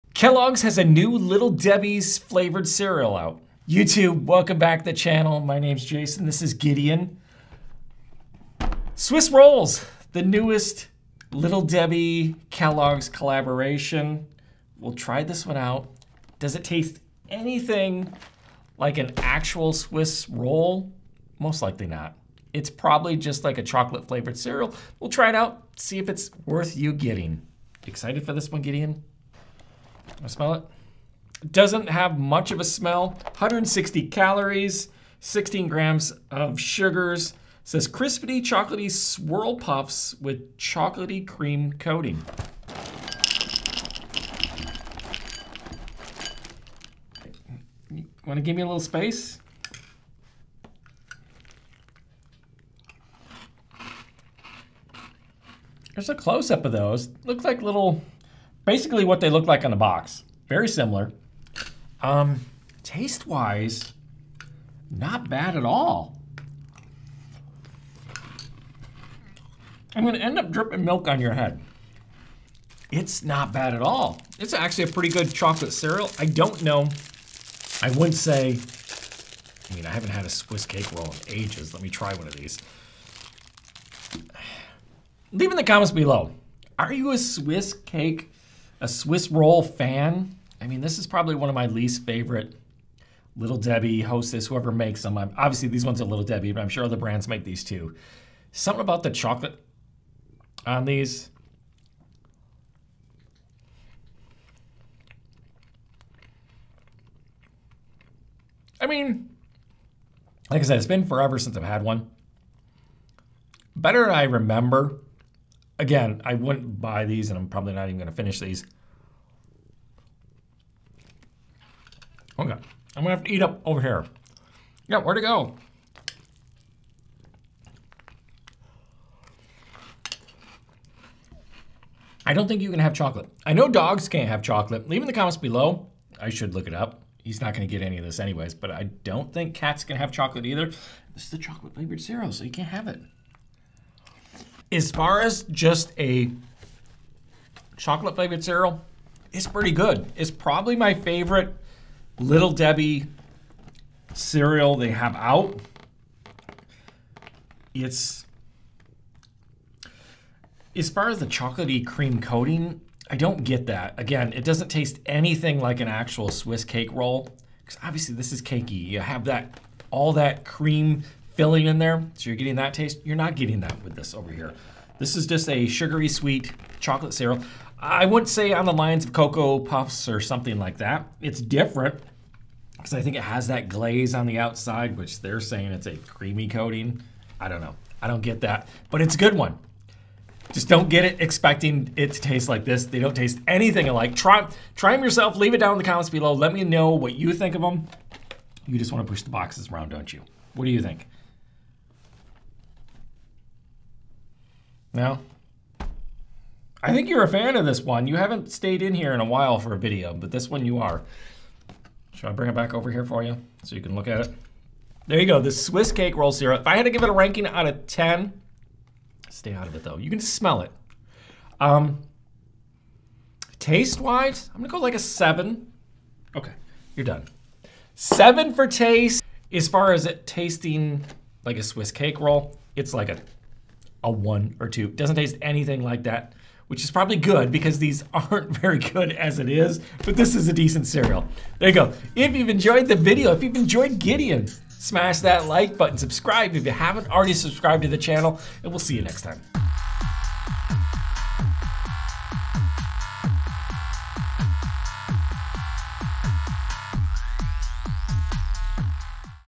Sweet Swiss Delight: Little Debbie Swiss Cake Roll Cereal Taste Test!
Dive into the delectable world of breakfast treats with us as we sample and savor Little Debbie's Swiss Cake Roll Cereal! In this video, we're crunching, munching, and sharing our thoughts on this delightful cereal inspired by the classic snack. Does it capture the essence of those beloved Swiss Cake Rolls? Join us on this indulgent breakfast journey to discover if it's a sweet morning sensation. Will it become your new breakfast favorite? Tune in to satisfy your cereal cravings! 🥄🍰🤤